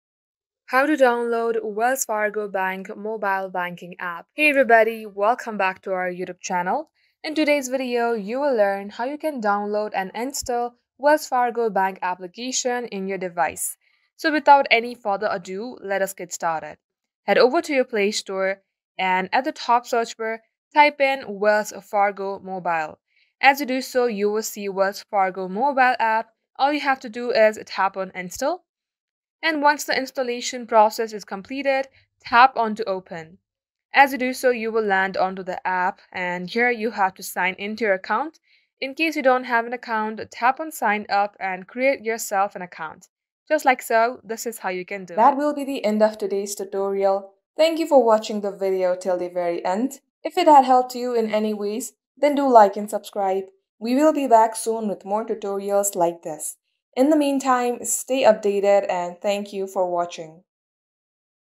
How To Download & Install Wells Fargo Bank Mobile (2024)
In this video, we provide a comprehensive guide on how to download and install the Wells Fargo Mobile app on your smartphone. Stay on top of your finances with one of the most popular banking apps available today. Whether you're a new user or simply updating to the latest version, our tutorial will help you through the process step-by-step.

What you'll learn in this video:
- How to find the Wells Fargo Mobile app in your app store
- Tips for downloading and installing the app successfully
- Features and benefits of using the Wells Fargo Mobile app for your banking needs

The Wells Fargo Mobile app makes banking convenient and secure, allowing you to manage your finances from anywhere at any time. Ensure you have the most updated version of the app to take full advantage of all its features.

Key Topics Covered:
- Downloading the Wells Fargo app
- Installing the Wells Fargo app on iOS and Android
- Features of the Wells Fargo Mobile app